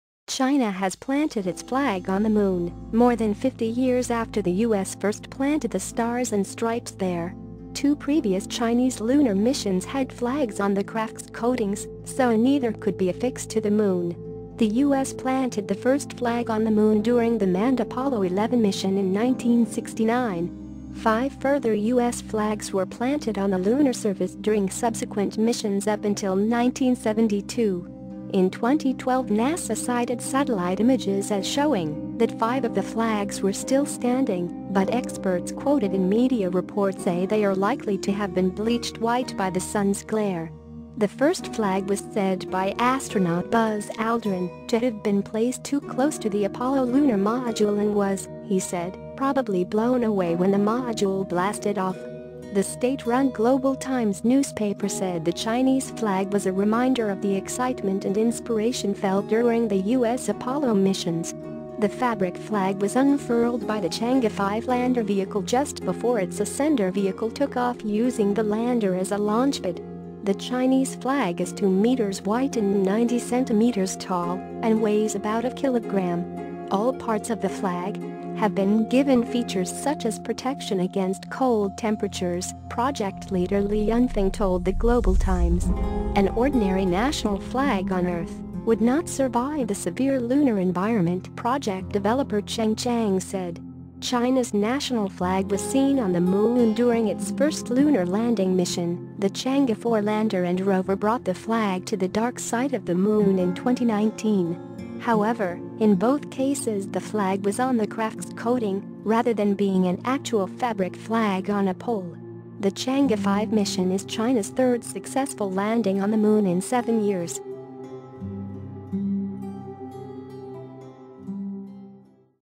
China becomes second nation to plant flag on the Moon
China has planted its flag on the Moon, more than 50 years after the US first planted the Stars and Stripes there.
Two previous Chinese lunar missions had flags on the crafts' coatings - so neither could be affixed to the moon.
The US planted the first flag on the Moon during the manned Apollo 11 mission in 1969. Five further US flags were planted on the lunar surface during subsequent missions up until 1972.
In 2012 Nasa cited satellite images as showing that five of the flags were still standing, but experts quoted in media reports say they are likely to have been bleached white by the sun's glare.
The first flag was said by astronaut Buzz Aldrin to have been placed too close to the Apollo lunar module and was, he said, probably blown away when the module blasted off.
The state-run Global Times newspaper said the Chinese flag was a reminder of the "excitement and inspiration" felt during the US Apollo missions.
The fabric flag was unfurled by the Chang'e-5 lander vehicle just before its ascender vehicle took off using the lander as a launchpad.
The Chinese flag is 2m wide and 90cm tall and weighs about a kilogram. All parts of the flag have been given features such as protection against cold temperatures, project leader Li Yunfeng told the Global Times.
"An ordinary national flag on Earth would not survive the severe lunar environment," project developer Cheng Chang said.
China's national flag was seen on the Moon during its first lunar landing mission,
The Chang'e-4 lander and rover brought the flag to the dark side of the moon in 2019.
However, in both cases the flag was on the crafts' coating rather than being an actual fabric flag on a pole.
The Chang'e-5 mission is China's third successful landing on the Moon in seven years.